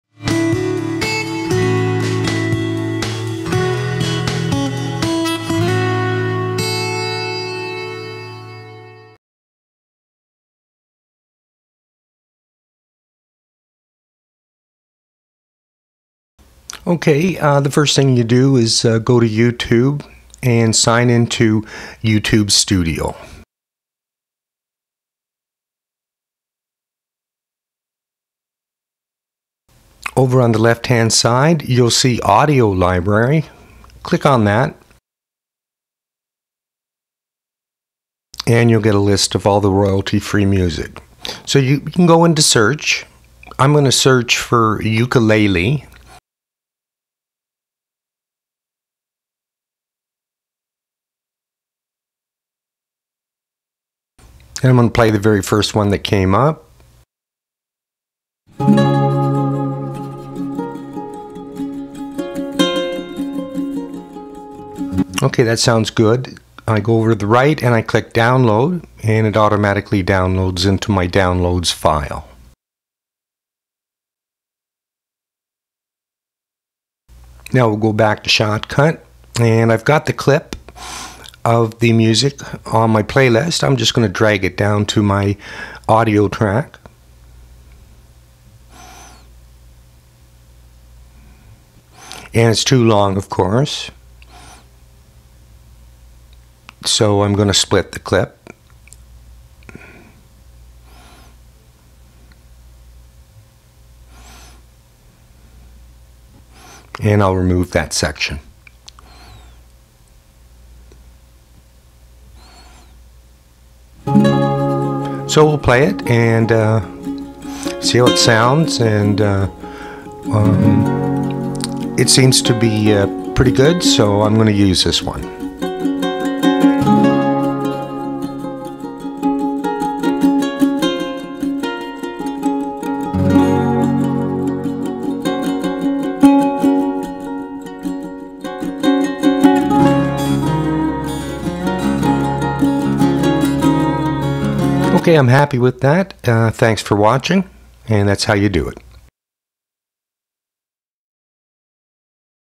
Shotcut - Add Royalty Free Music
Go to YouTube Studio, Click on the Audio Library.